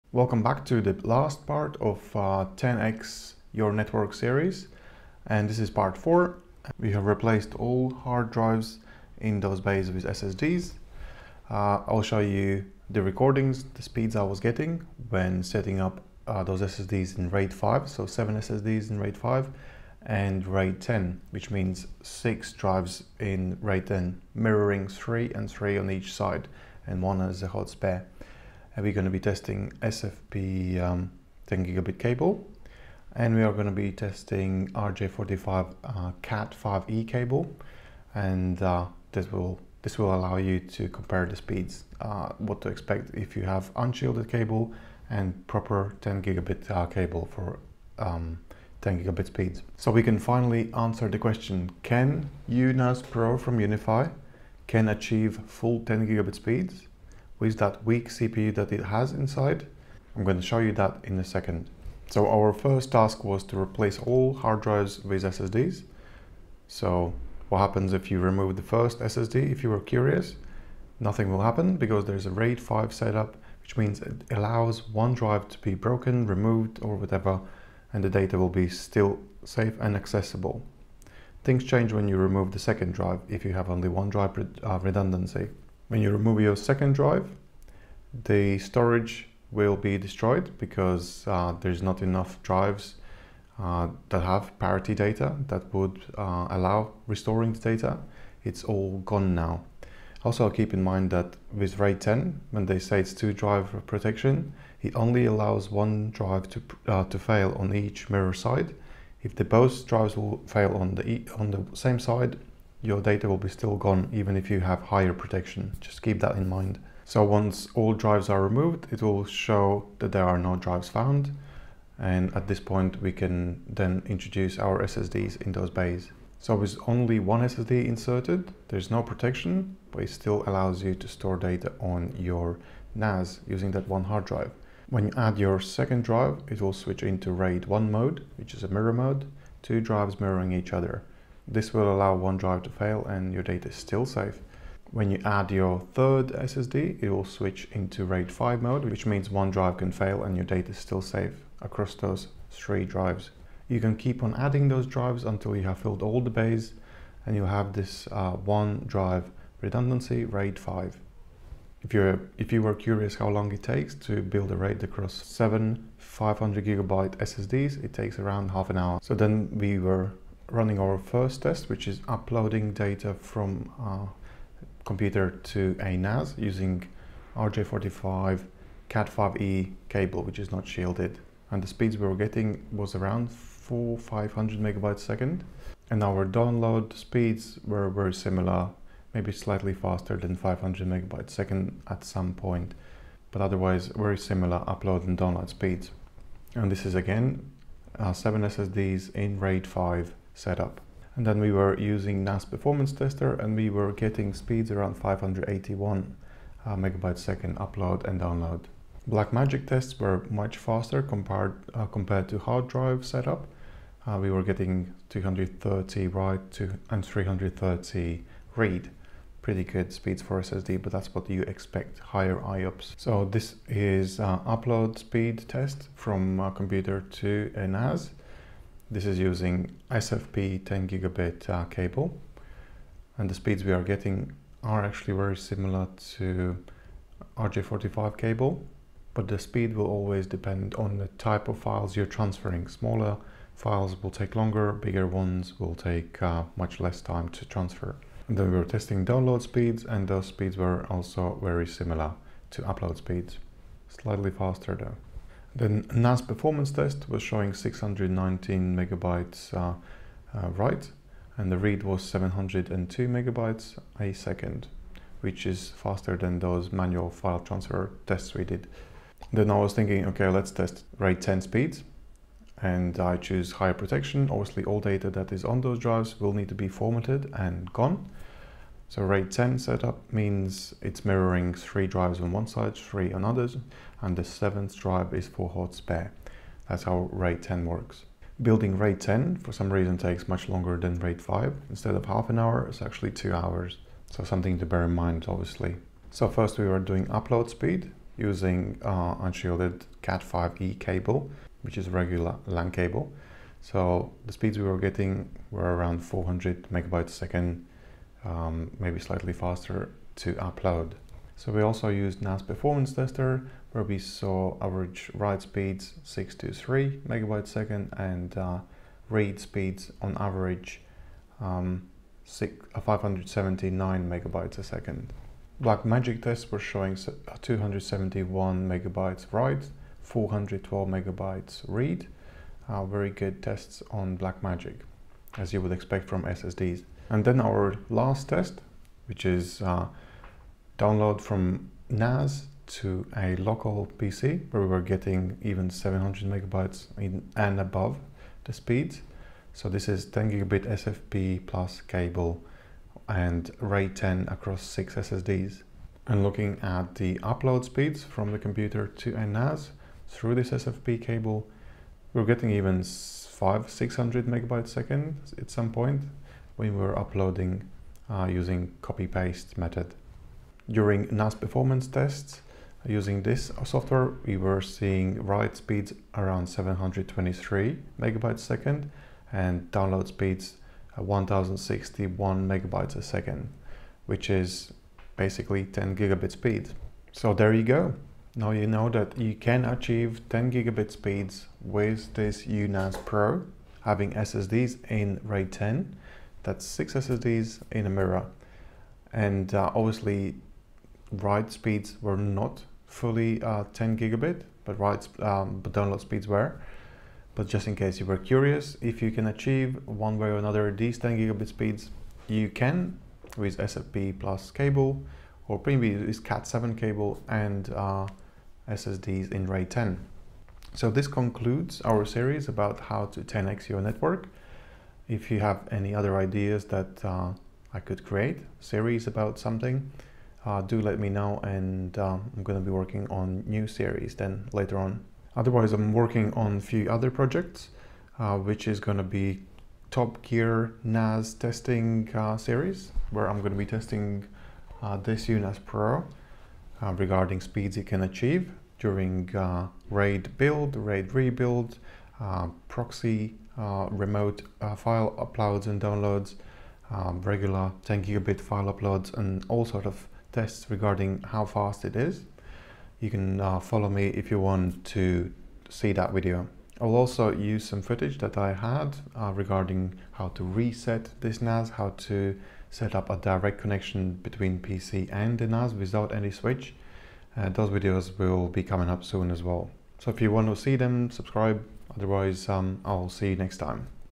10X Your Network Speed: Unifi uNAS Pro- 10Gbit speed with SSDs, RAID 10, and SFP+ (Part 4)
Welcome to the final part of the "10x Your Network" series! In this video, we explore how to replace hard drives with SSDs, configure RAID 5 and RAID 10, and test network speeds using different cables. Learn how to optimize your NAS setup and achieve blazing-fast 10GB speeds. Stick around for future plans and upcoming video series announcements!

Timestamps:
00:00 - Introduction to the series (Part 4)
00:06 - Replacing hard drives with SSDs
00:16 - RAID 5 and RAID 10 setup and explanation
00:31 - Testing network speeds with different cables
01:04 - Effects of removing drives in RAID configurations
02:07 - SSD configurations and RAID modes
02:55 - Test 1: Upload and Download with RJ45 Cat 5e cable
03:23 - Test 2: NAS Performance Test
03:29 - Test 3: Blackmagic Test
03:52 - Test 4: Upload and Download
04:06 - Test 5: NAS Performance Test
04:34 - Setting up RAID 10
05:02 - Test 6: Upload and Download with RJ45 Cat 5e cable
05:19 - Test 7: NAS Performance Test (Cat 5e cable)
05:27 - Test 8: Blackmagic Test
05:46 - Test 9: Download with SFP+ cable
06:10 - Test 10: NAS Performance Test (SFP+ cable)
06:52 - Concluding the series
07:14 - Future plans and upcoming videos